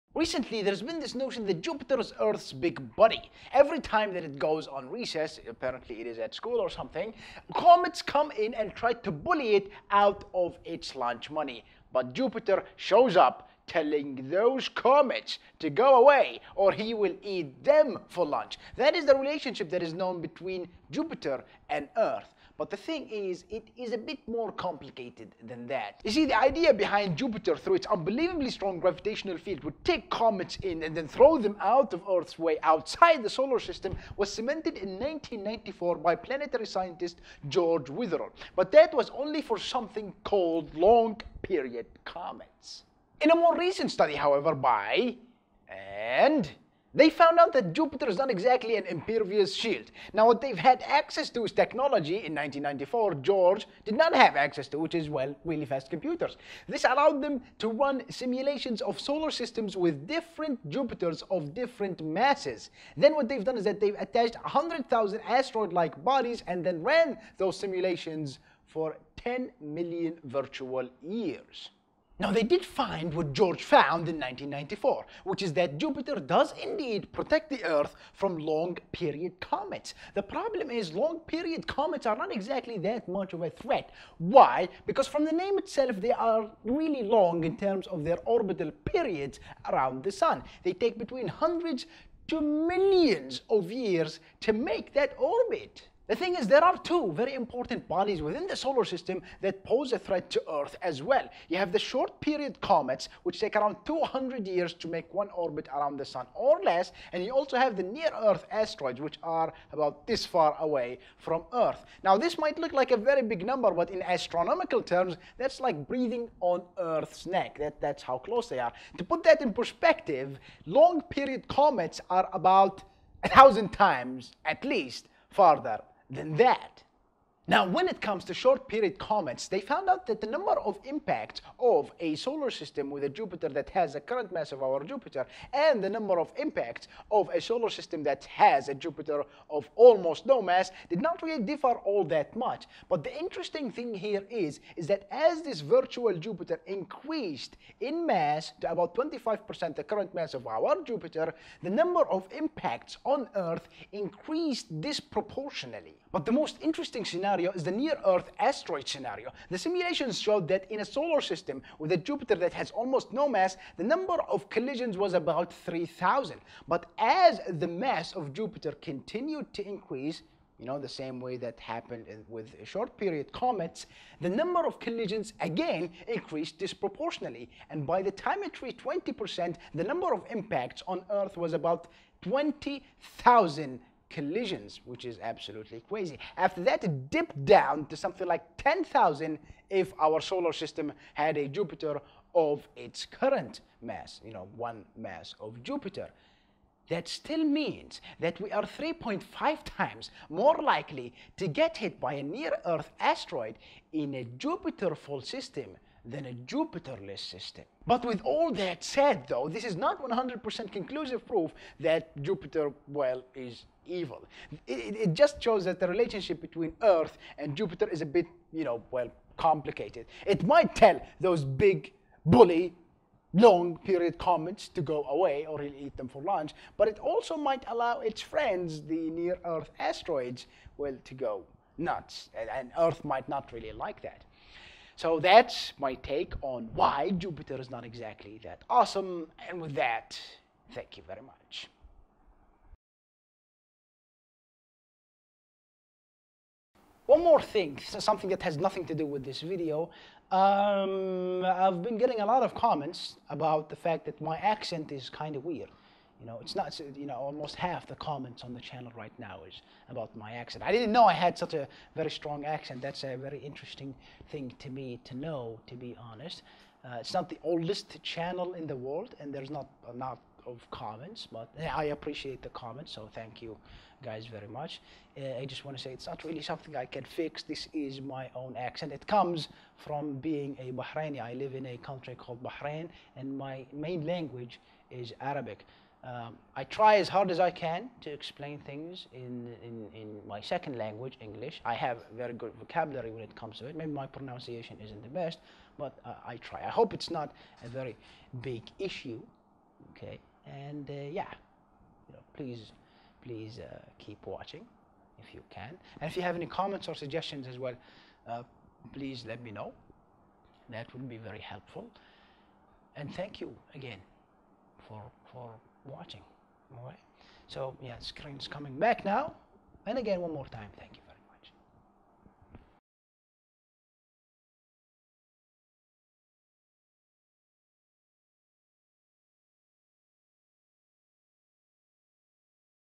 Why Jupiter is not That Awesome
Recently, Jupiter has been credited with being an impenetrable shield against the threats of comets to earth, but is Jupiter really as awesome as it is described?

(Study By Jonathan Horner/Barrie Jones)

(Difference between long and short period comets)

(Shows how Jupiter is protecting earth)